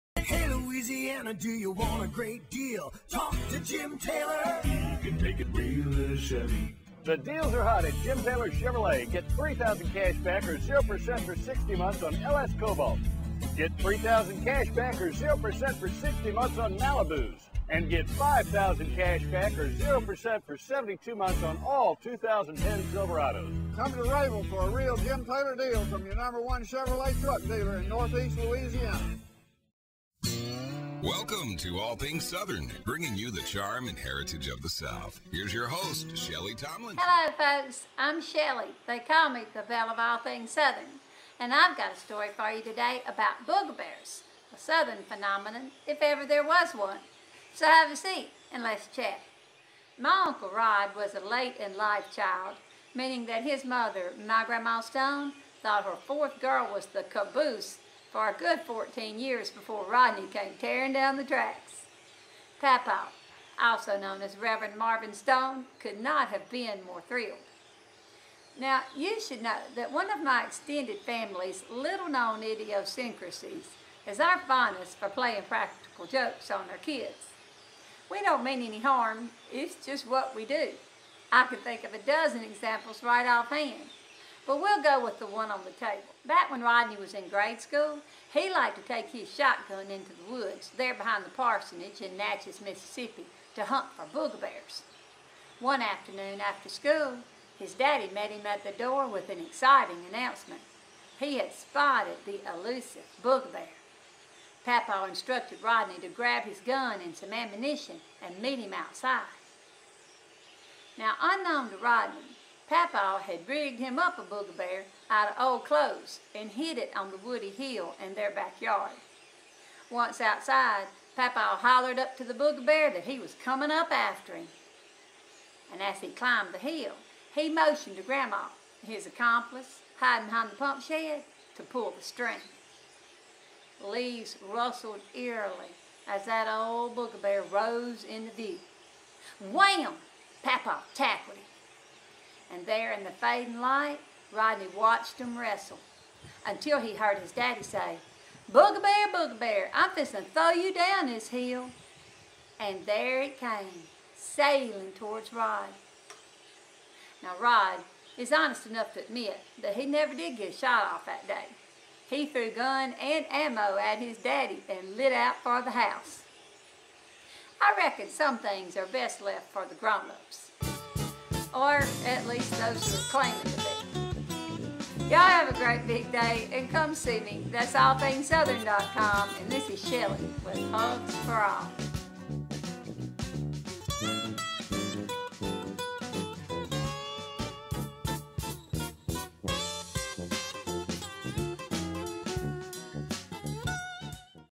Big Kids and Booger Bears
Shellie Rushing Tomlinson, Belle of All Things Southern, shares a classic Booger Bear story from her family.